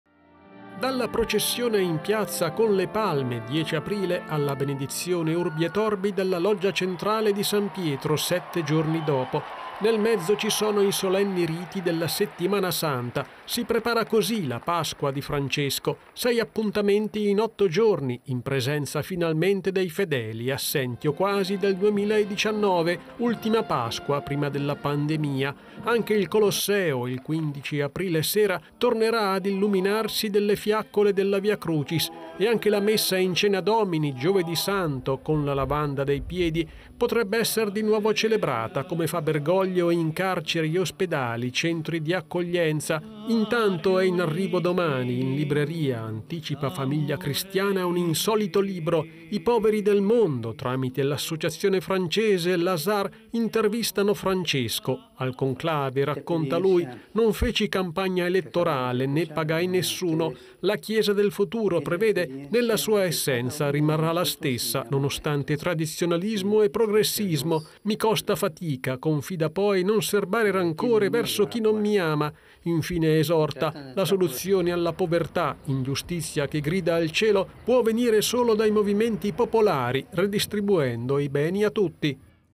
Papa Francesco, tornano in presenza celebrazioni Settimana Santa. In uscita libro-intervista
Dopo le limitazioni dovute alla pandemia, tornano in presenza anche le celebrazioni della Settimana santa, presiedute da Papa Francesco.
Servizio di Paolo Fucili